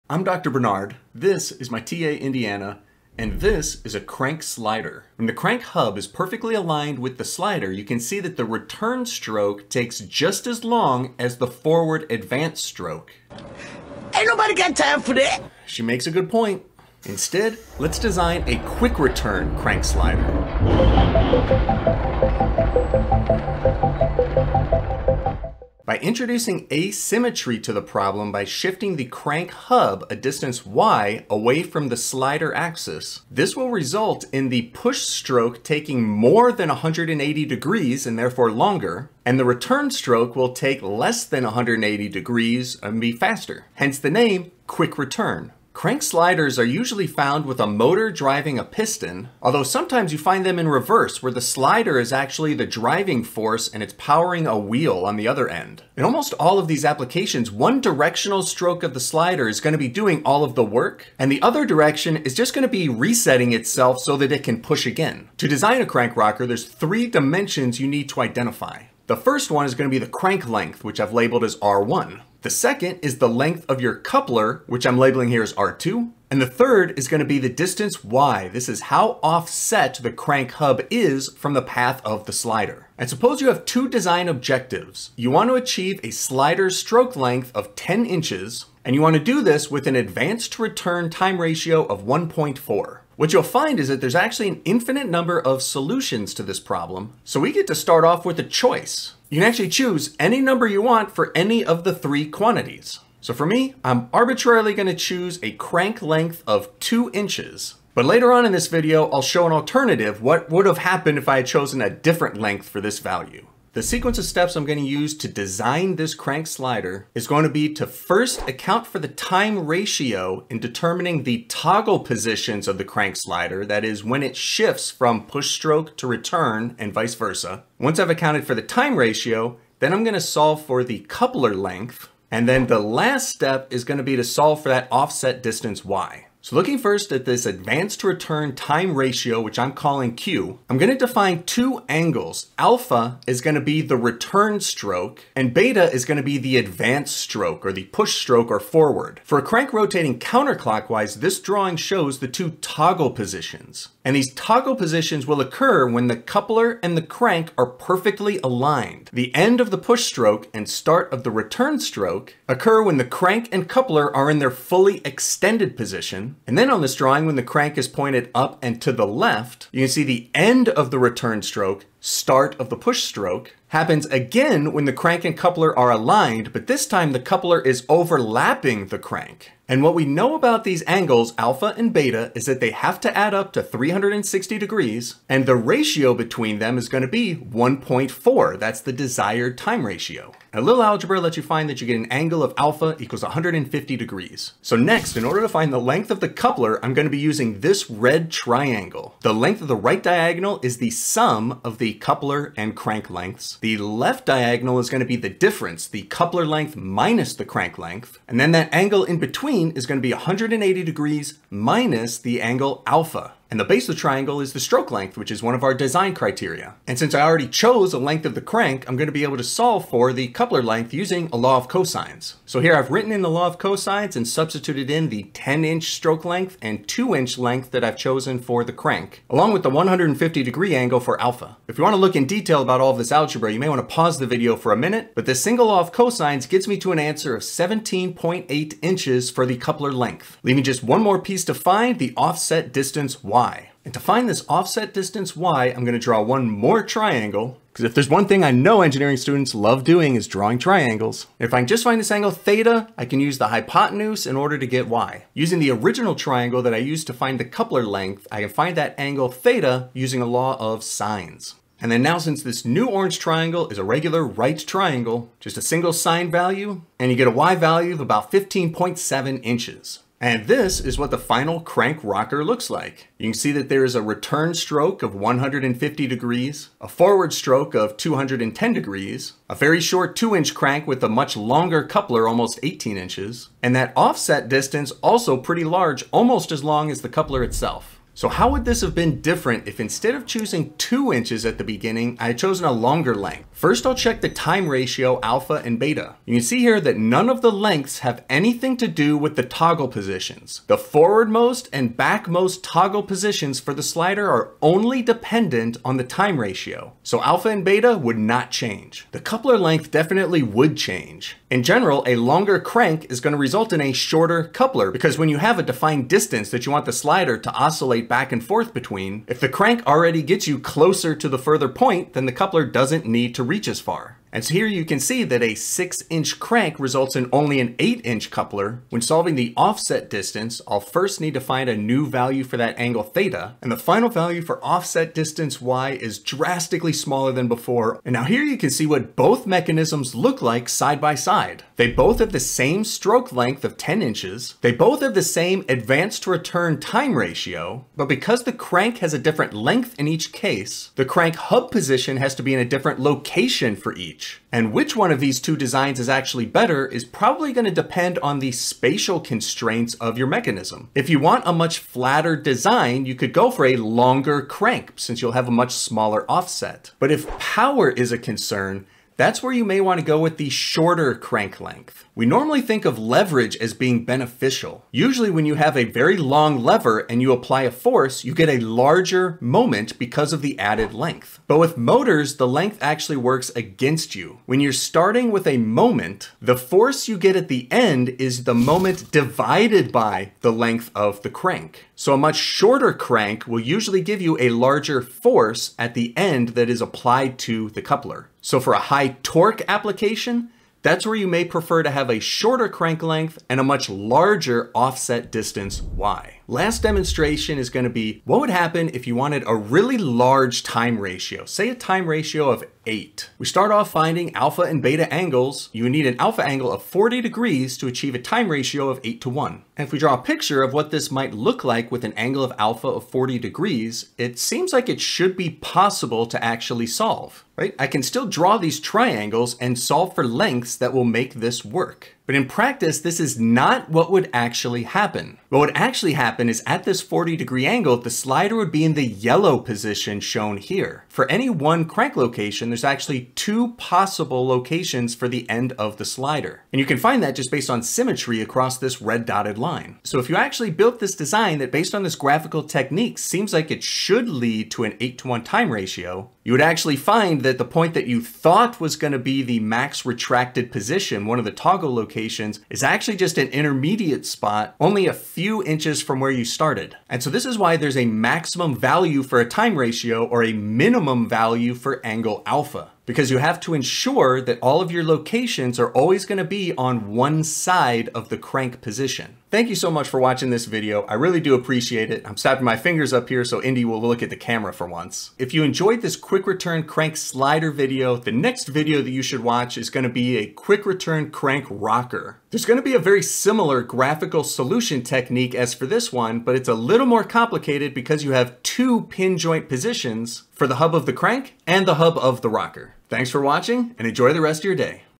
Design a Quick Return Crank Slider for given Time Ratio
An offset slider crank is designed as a Quick Return Crank Slider to satisfy a given quick return time ratio and stroke length.

Design a Quick Return Crank Slider requires solving for crank, coupler, and offset lengths. Crank Slider Toggle Positions as alpha/beta crank angles are solved first using the advance return time ratio, then the coupler length, and finally the offset distance are solved next using trigonometry, law of sines, and law of cosines.

I end with discussion of the maximum possible time ratio, since the graphical technique demonstrated can fail in a non-obvious way when the time ratio is too large.

TIMECODES

0:00 Quick Return Mechanisms
1:05 Quick Return Crank-Sliders
2:16 Using Time Ratio
3:42 Finalizing the Design
5:38 How to choose your crank length
6:45 Maximum Time Ratio